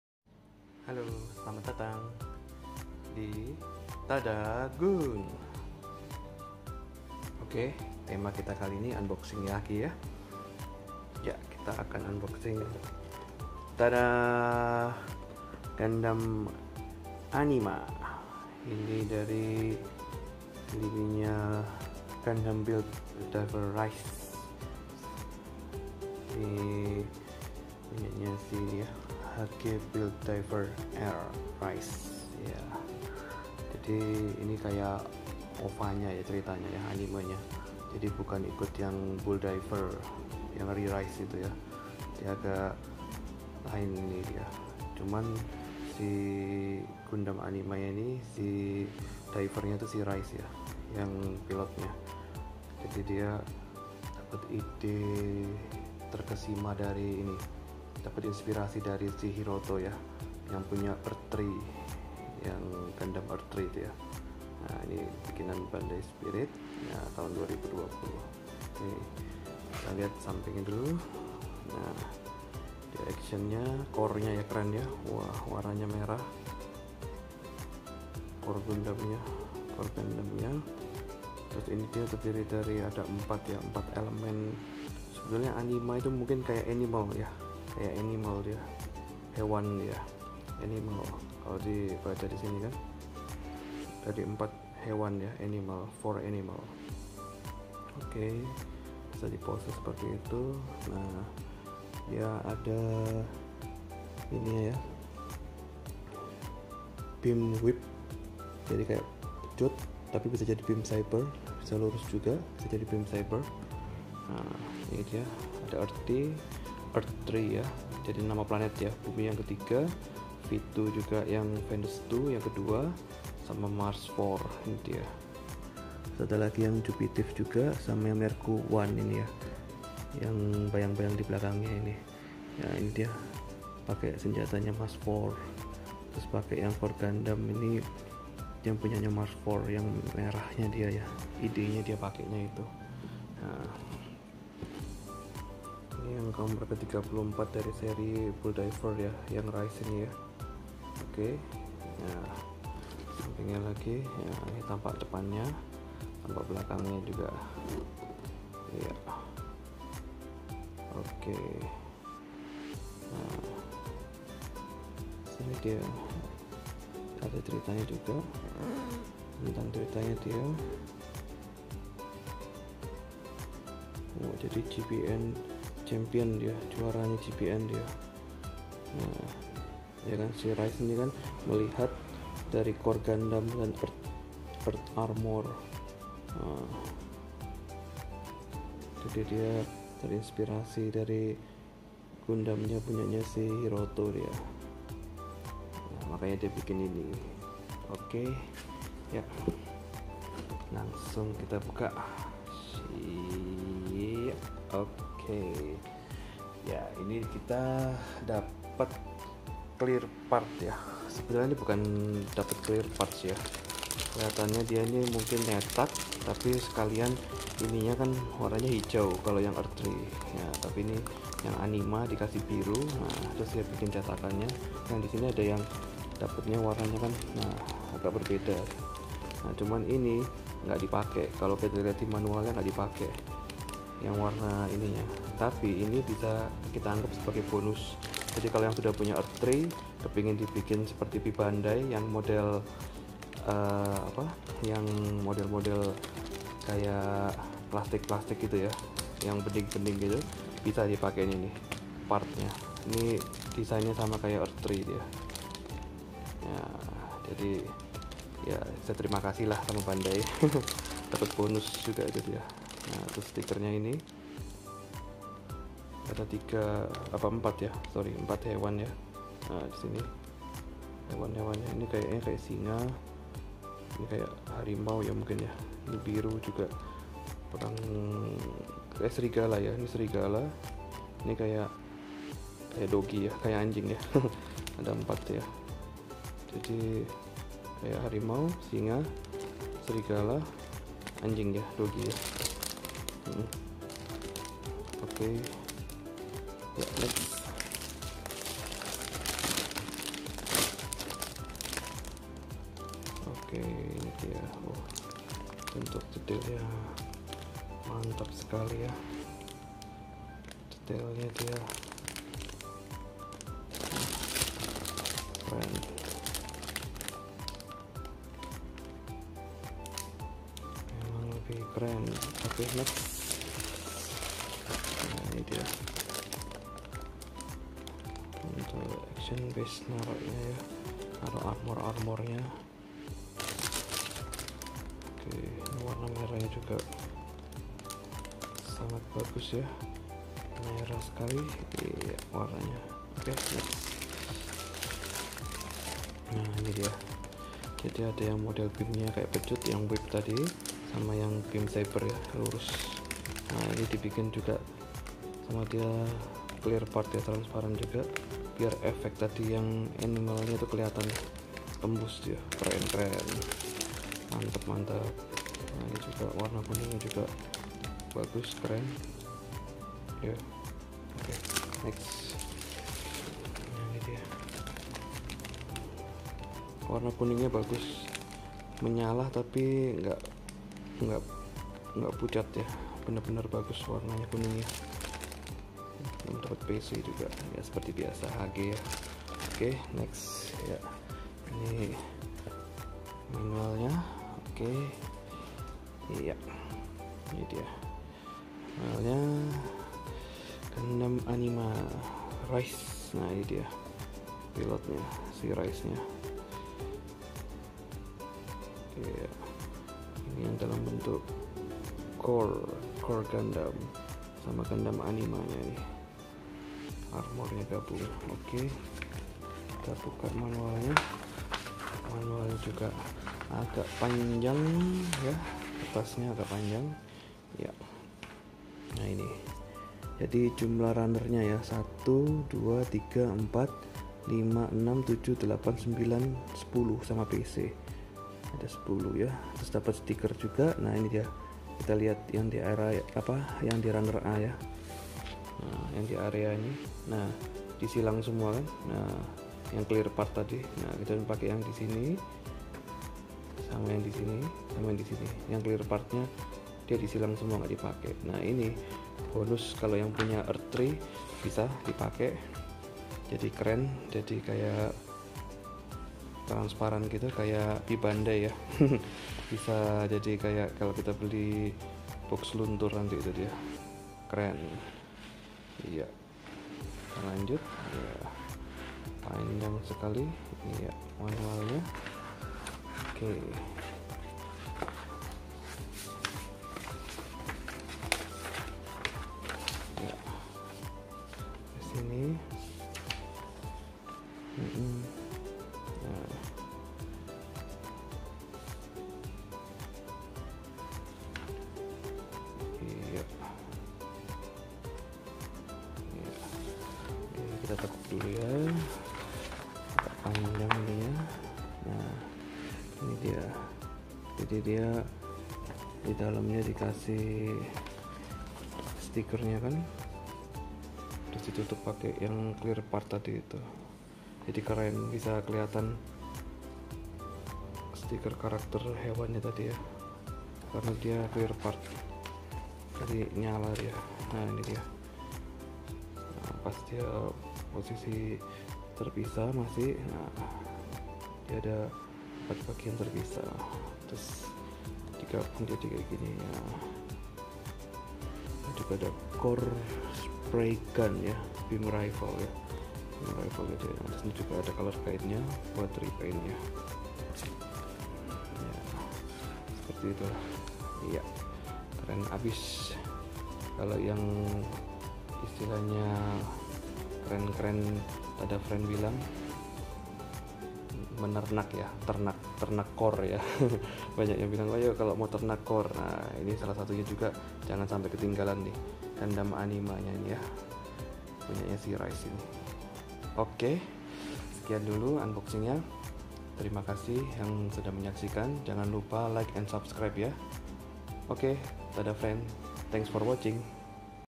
HGBD:R • Gundam Anima (Rize) • TadaaGun • Unboxing
Halo TadaaFren...

TG kali ini akan unboxing dari lini HG Build Diver Rerise yaitu Gundam Anima...
Gundam Anima (mungkin nama Anima diambil dari nama animal / hewan) ini adalah Gunpla milik Diver bernama Rize, dari anime side story Gundam Build Diver Rize...
Di dalam box juga terdapat runner clear part yang bisa digunakan untuk Gundam E3 ya...
bentuk Gundam Anima keren abis dan terdiri dari 4 element kekuatan hewan yaitu harimau, singa, serigala dan anjing.

Ok selamat menyaksikan ya... ;)